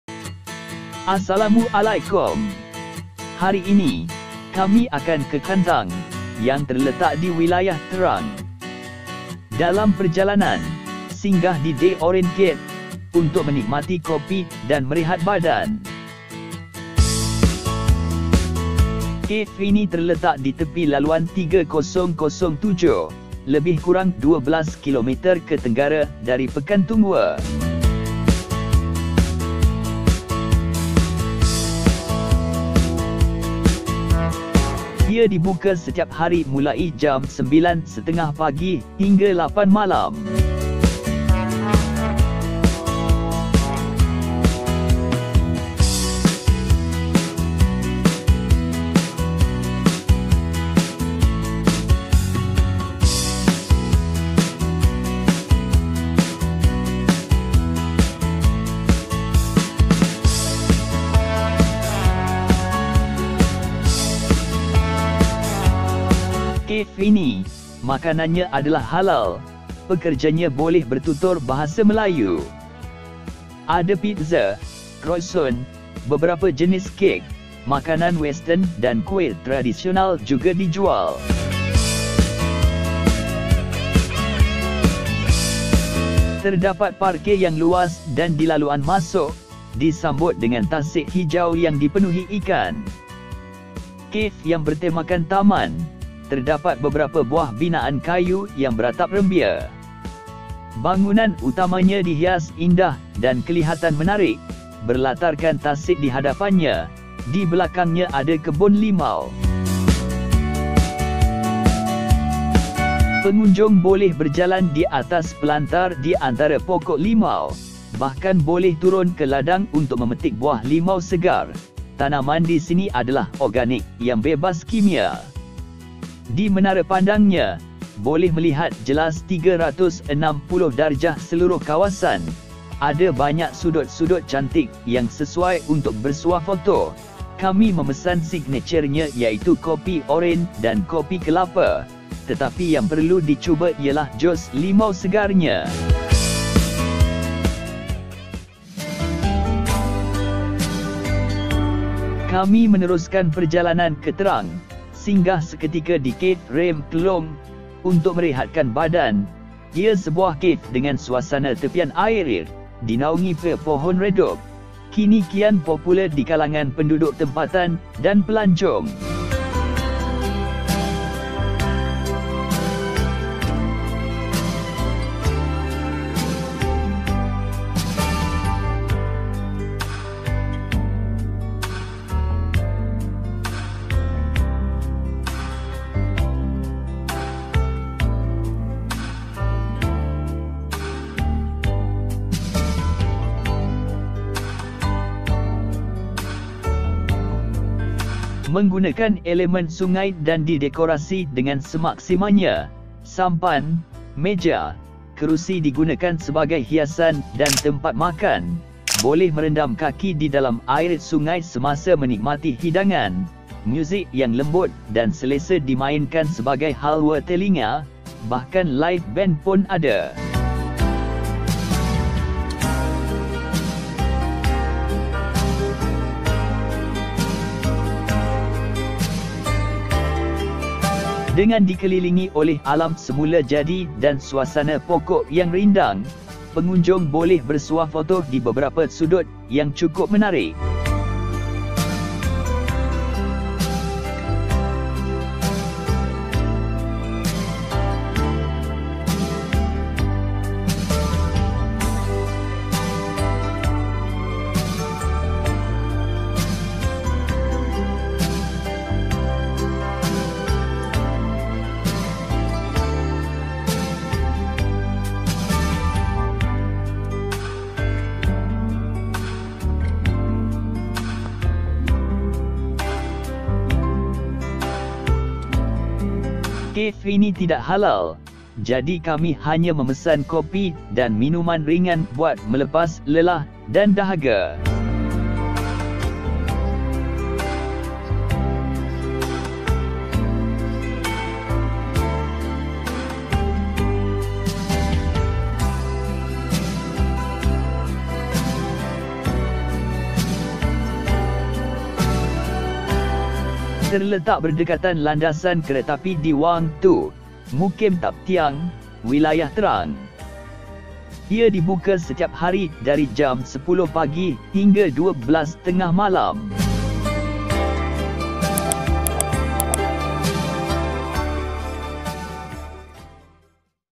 Memburu Café Hipster Di Satun & Trang
Dari Wang Klian Menuju Ke Kantang, Singgah menikmati kopi untuk menyegar badan dan menikmati suasana
1. The Orange Cafe, Thung Wa
2. Rim Klong Cafe, Trang